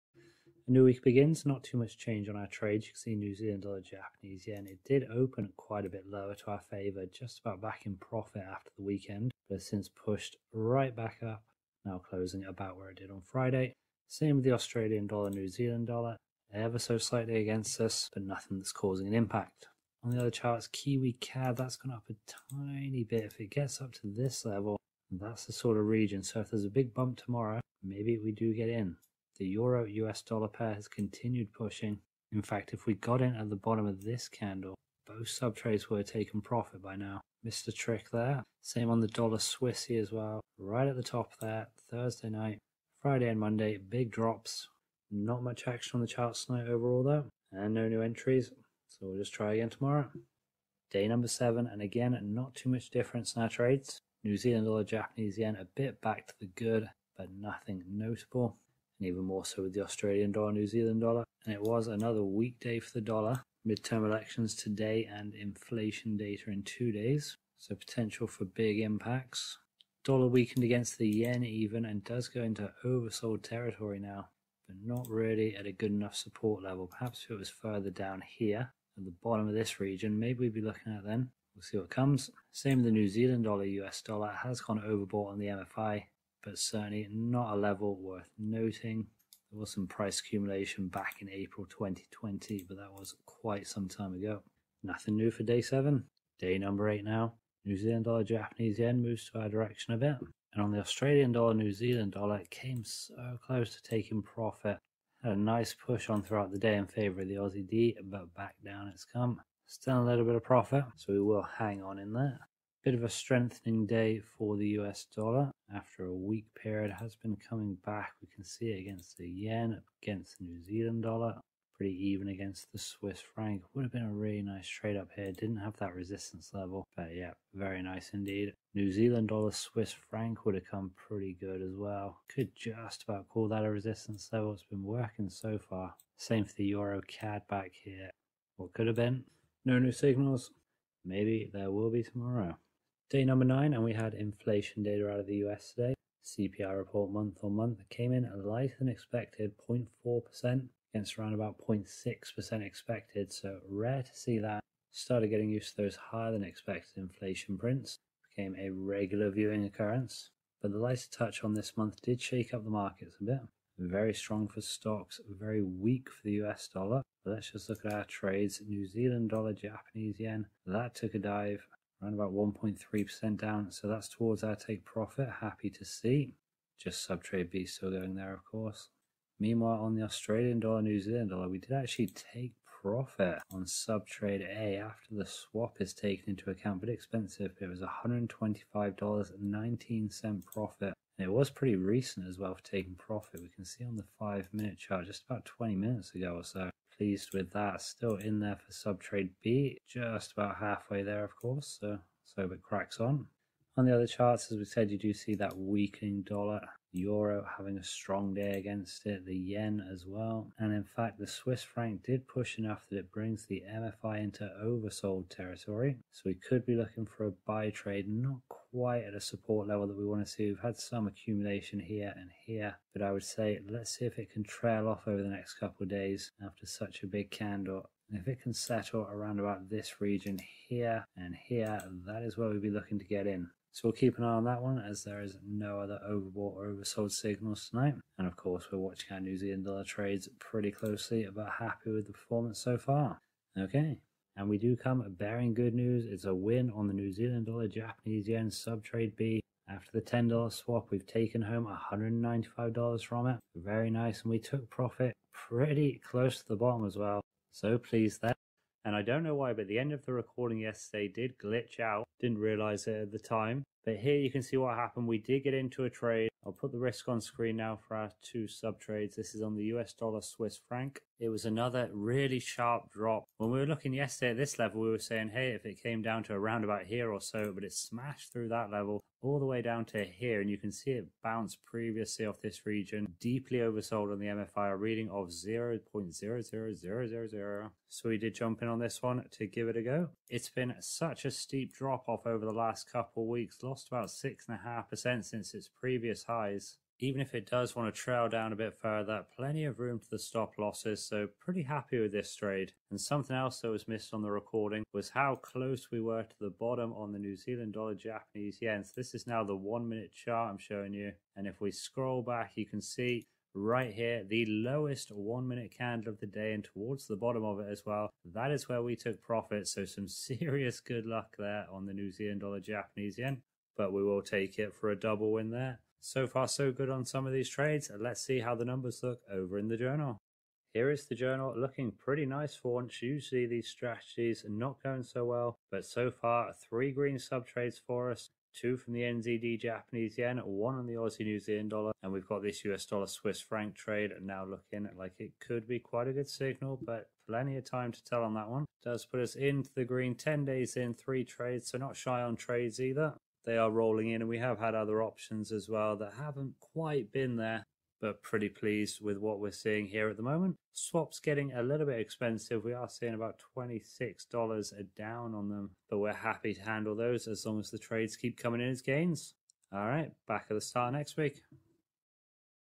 MFI Support & Resistance D1 Strategy - Part 2 | Trading Highlights
STRATEGY
As the strategy name suggests this incorporates two indicators for our entry signals. They are the technical indicator of the money flow index (MFI) and the price structure indicator of support and resistance levels. First off we’ll look at the MFI indicator. It’ll be input with 6 inputs so a brief time frame, but we’ll keep the standard upper and lower levels of 80 and 20. If the MFI is reading above 80 then it’ll be sell trade potential for us, or below 20 will be a possible buy trade.
Before jumping into the trade though we’ll want a confirmation from the price structure. That will be support and resistance levels, where a support level will put us onto a buy trade, and a resistance level a buy trade. A technical indicator and a price structure indicator working in tandem to get us into our trade.
Once we are in the trade we’ll look to the Average True Range (ATR) indicator to help us out. Our trade will be split into two sub-trades so we’ve got a mixture of risk moves going on. Sub-trade A will be a 1.5x ATR stop loss and a 1x ATR take profit. Sub-trade B will be a wider stop loss and take profit, going a 2x ATR stop loss and 2x ATR take profit. The extra space will hopefully help us out where we get in at a support and resistance level and it runs just that little bit further. Then trade your asset of choice.